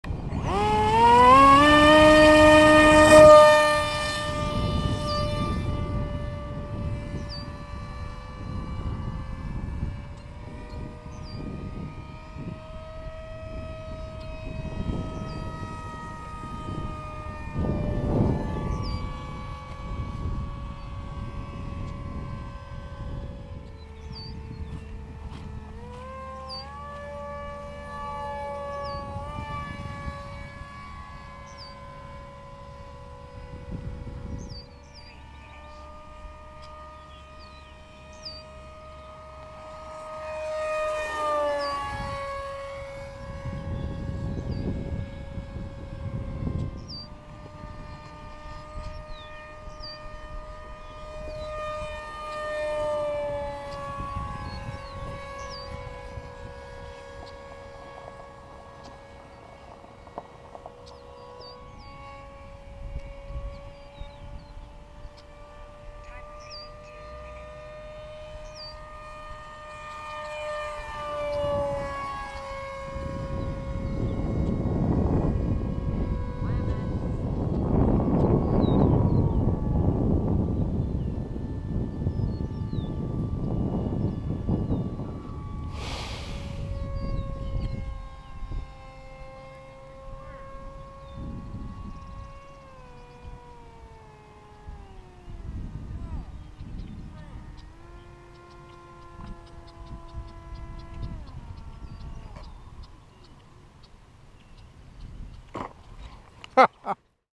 MEMORIAL NIGHTHAWK
MEMORIAL Day flight of my F-117 NIGHTHAWK.
This is a modified 80% RCP Nighthawk v2.